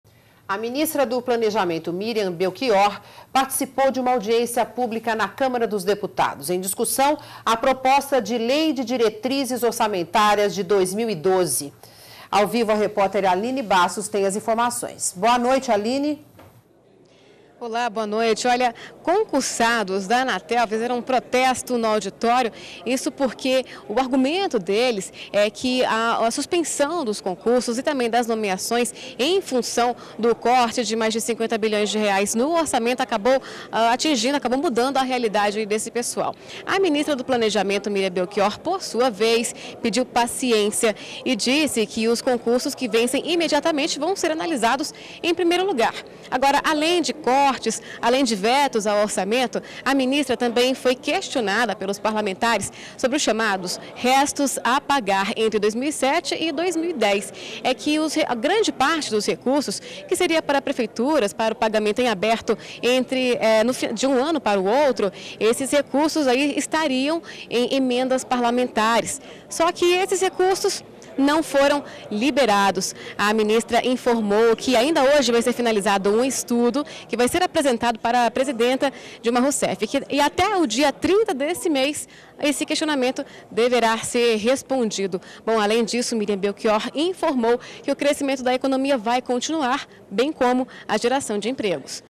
Miriam Belchior participa de audiência pública sobre Lei de Diretrizes Orçamentárias 2012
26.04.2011 - NBR Notícias 19h30: A ministra do Planejamento, Miriam Belchior, participou de uma audiência pública, na Câmara dos Deputados. Está em discussão a proposta de Lei de Diretrizes Orçamentárias de 2012.